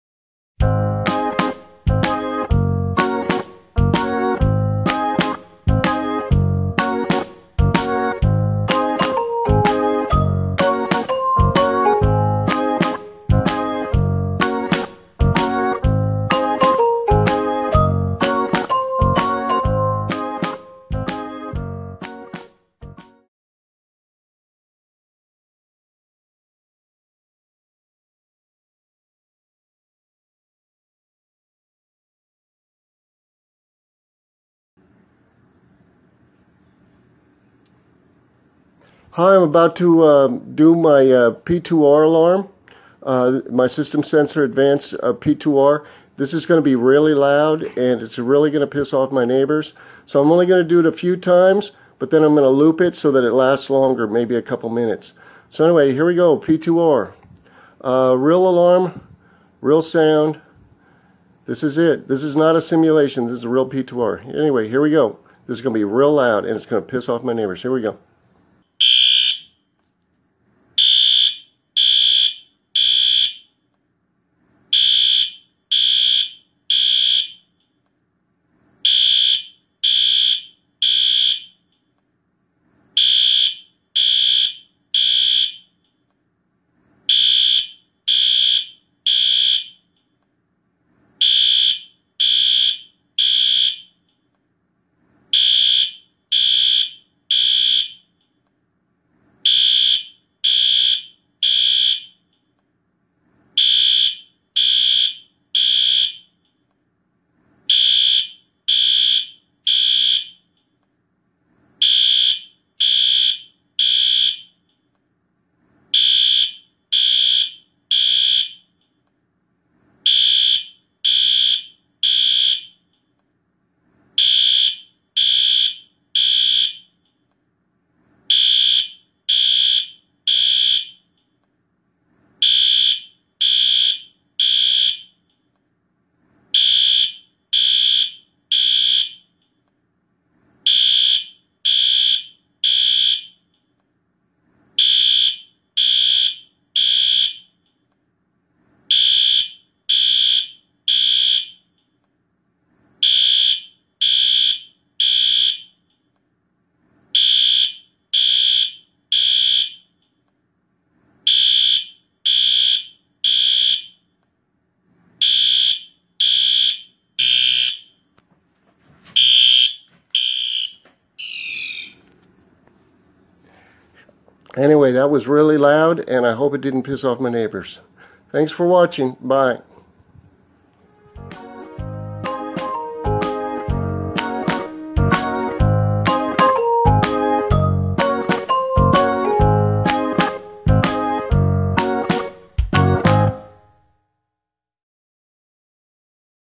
System Sensor P2R Fire Alarm
This is NOT a simulation. This is my real System Sensor SpectrAlert Advance P2R at full volume. My sincere apologies to my poor neighbors.